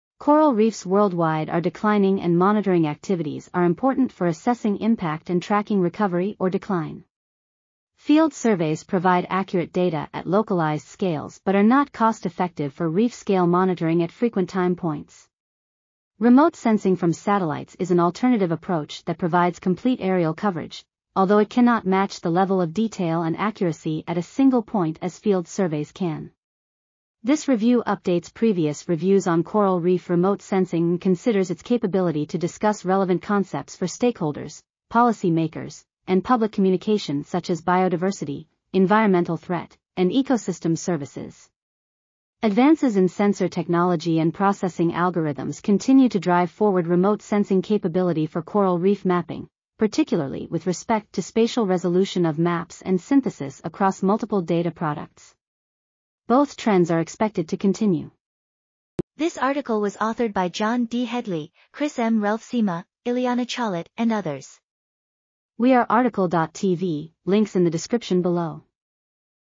Remote Sensing of Coral Reefs for Monitoring and Management: A Review | RTCL.TV
### Keywords ###

### Article Attribution ###
Title: Remote Sensing of Coral Reefs for Monitoring and Management: A Review
Authors: John D. Hedley, Chris M. Roelfsema, Iliana Chollett, Alastair R. Harborne, Scott F. Heron, Scarla Weeks, William J. Skirving, Alan E. Strong, C. Mark Eakin, Tyler R. L. Christensen, Victor Ticzon, Sonia Bejarano ,and Peter J. Mumby

### Video Timestamps ###
0:00:00 - Summary
0:00:58 - Title
0:01:05 - Tail
0:01:09 - End